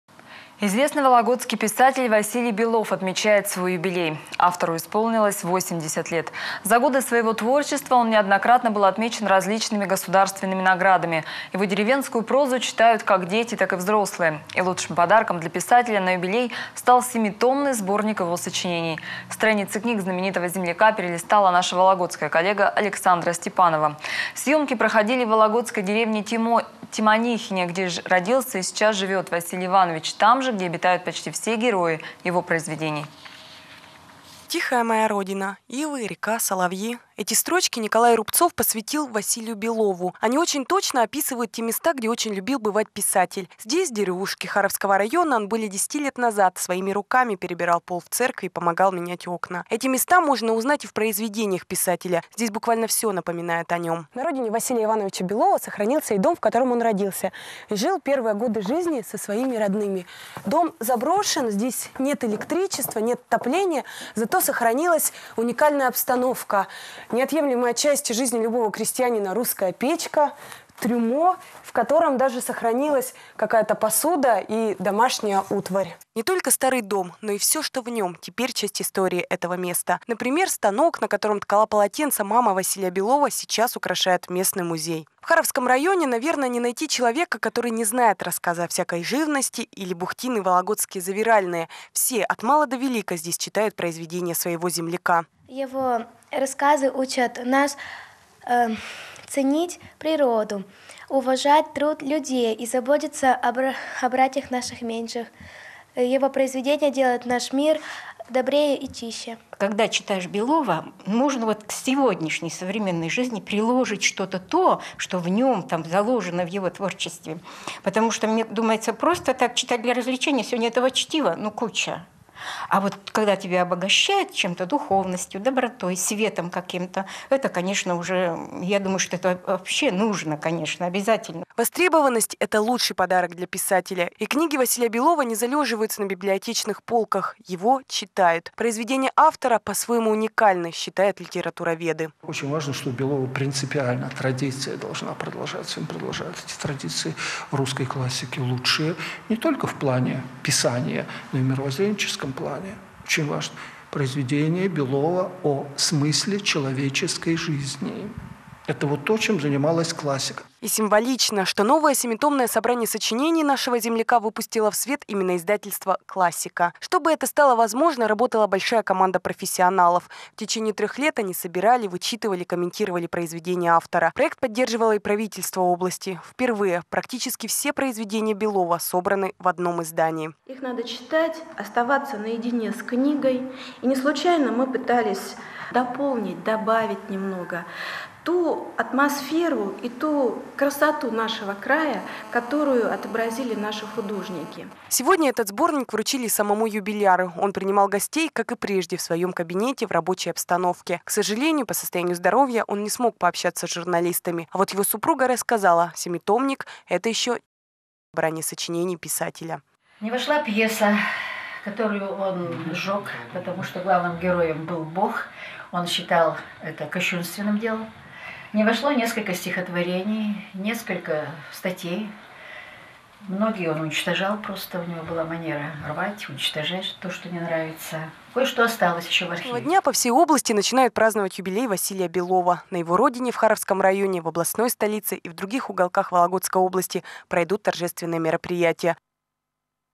Вологодский писатель Василий Белов отметил 80 лет
За годы своего творчества он неоднократно был отмечен различными государственными наградами. Его деревенскую прозу читают как дети, так и взрослые. И лучшим подарком для писателя на юбилей стал семитомный сборник его сочинений. Василий Иванович родился и живет сейчас в вологодской деревне Тимонихе, там же, где обитают почти все герои его произведений.